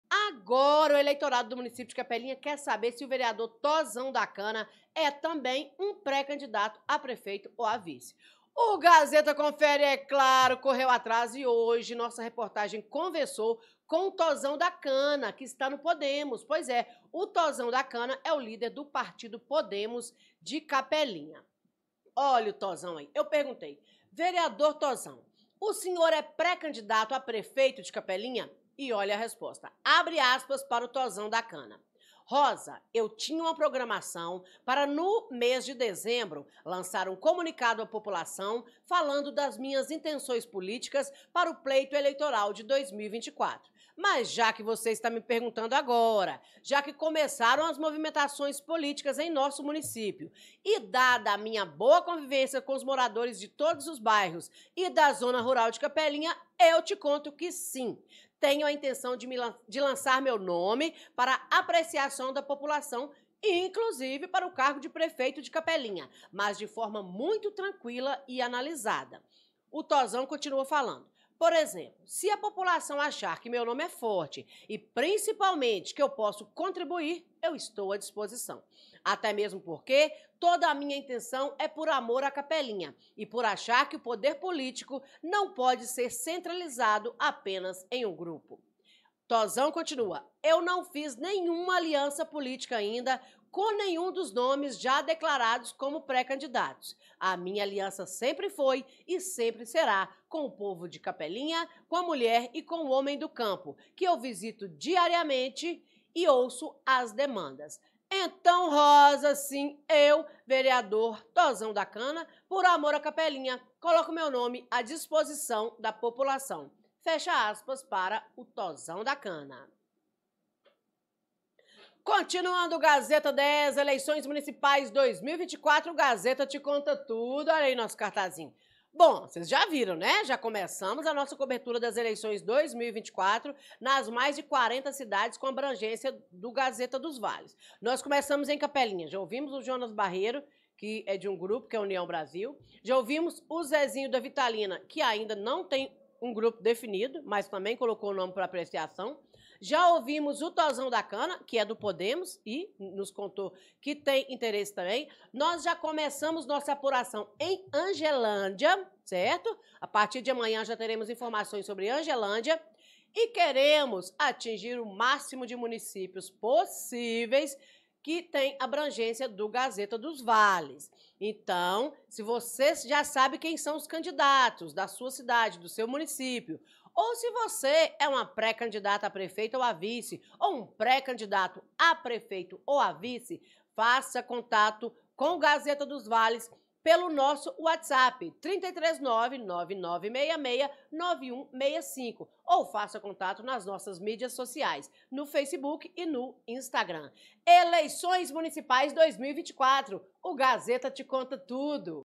TOZÃO DA CANA, DO PODEMOS, COLOCA NOME PARA APRECIAÇÃO POPULAR VISANDO A PREFEITURA DE CAPELINHA
Continuando as apurações sobre os preparativos para as Eleições Municipais 2024, o GAZETA 10 de 16/11/23 conversou com o vereador Tozão da Cana. Confira.

Gazeta Dos vales
O Portal de Notícias de Capelinha e região que mantém você atualizado sobre tudo que acontece no dia a dia da sua cidade